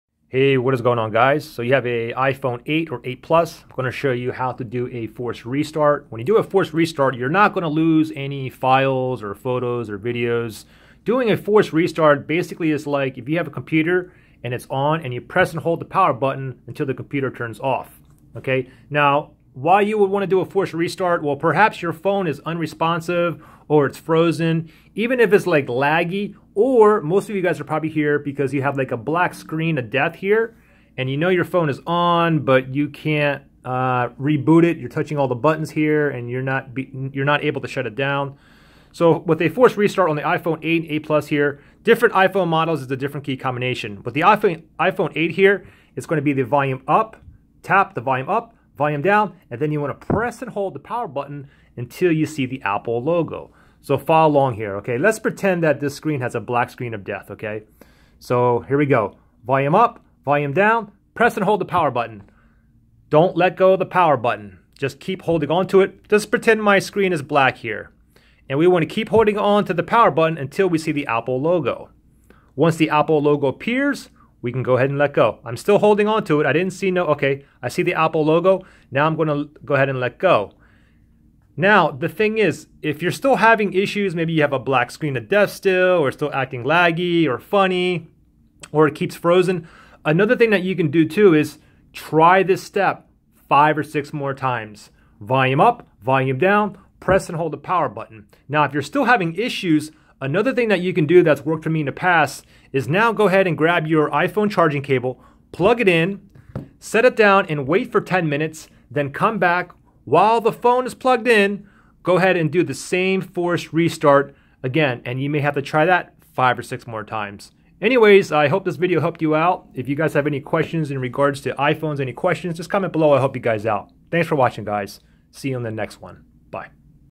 iPhone 8 or 8 Plus: How to do a Forced Restart
I show you how to force a restart (forced restart) on your iPhone 8 or 8 Plus. Forcing a restart can solve issues such phone becoming laggy, black screen, unresponsive, etc.